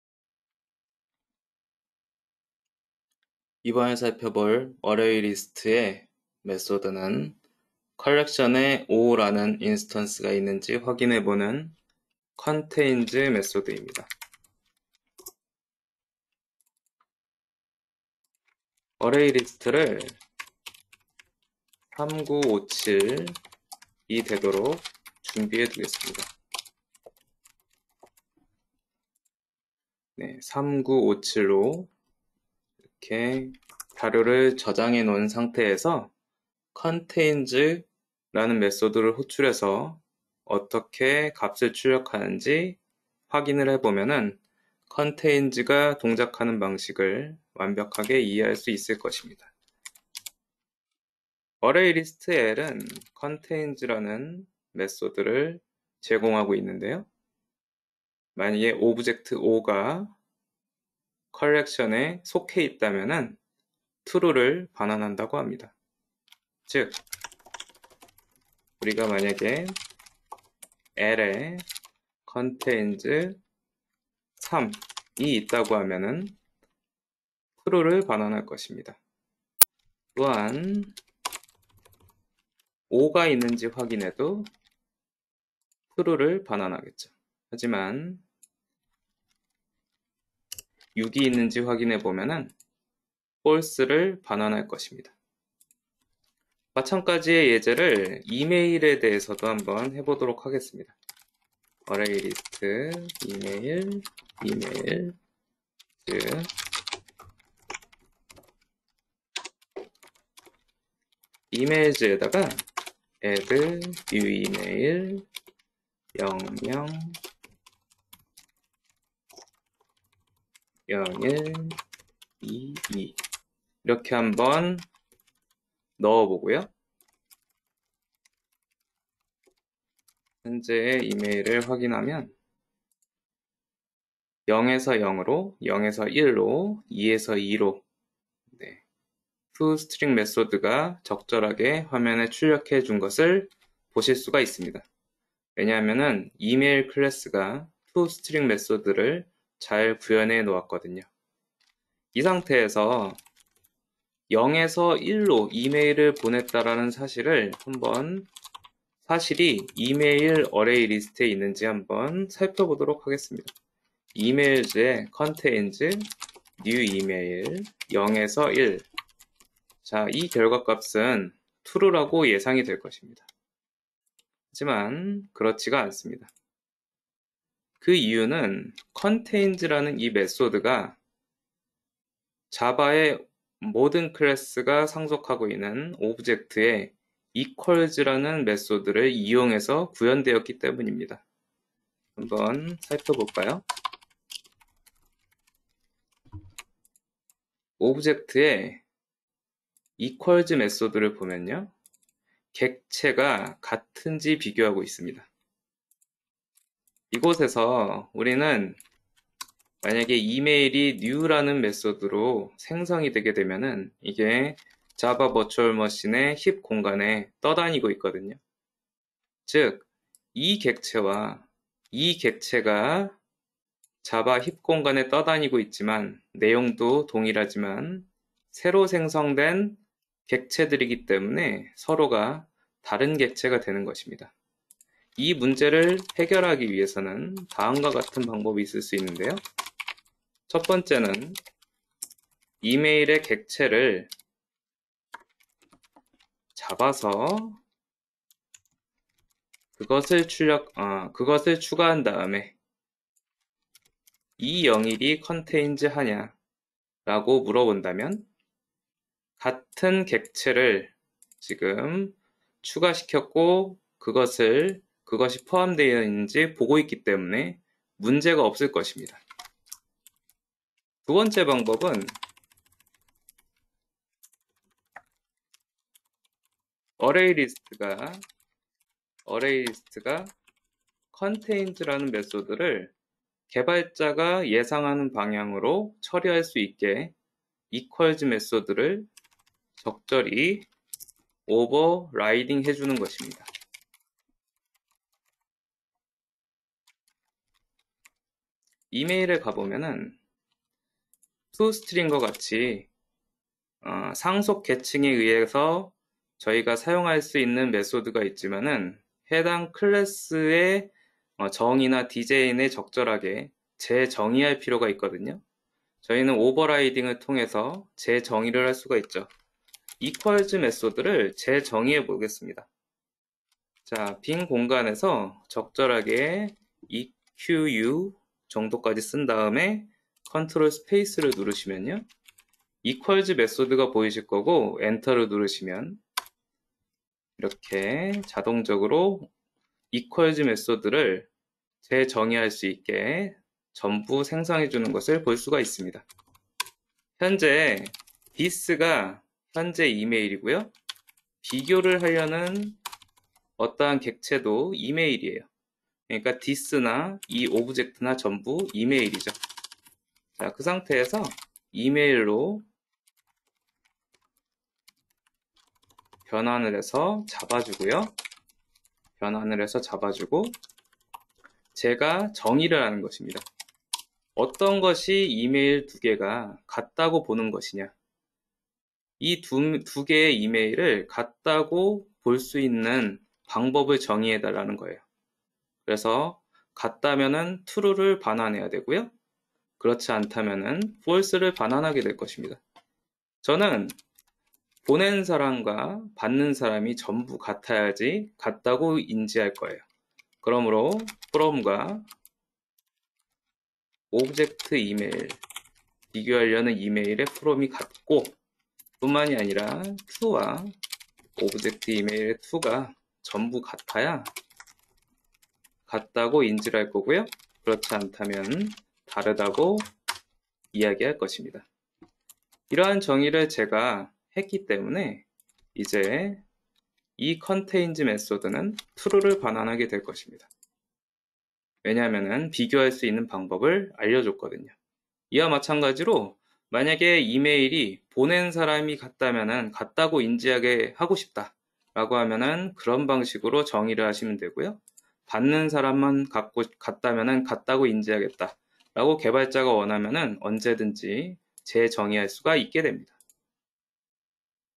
데이터 분석 3강 - ArrayList - contains
본 영상은 2021-2 KMOOC-데이터엔지니어링 강의의 원본이며, 강좌 오픈 이후에는 질의 응답 채널으로 이용 예정입니다. 각 Topic에 질문 해주시면 가능한 답은 달아드리도록 하겠습니다.
총 11강
구성
- ArrayList 연산
- MyArrayList - 배열 기반 List interface 구현
- LinkedList 연산
- MyLinkedList - 참조 기반 List & Queue interfaces 구현
- HashSet/HashMap 연산
- Lambda Expression
- Stream - Open JDK 15에서 이용가능한 거의 대부분의 연산을 다루려고 노력함